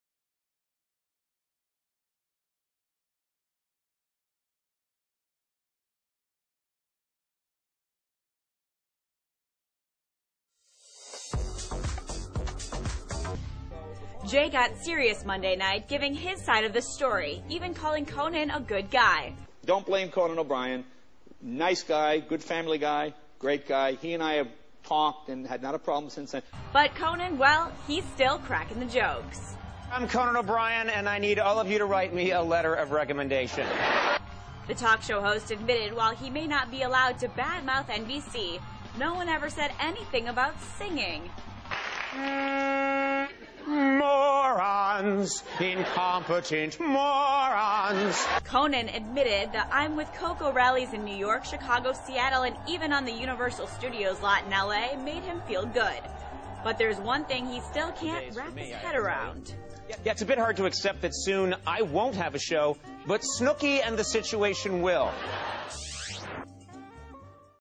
Conan O'Brien Supporters Hold I'm With Coco 9 Rally
In frnt of NBC5 Building on Michgan Av.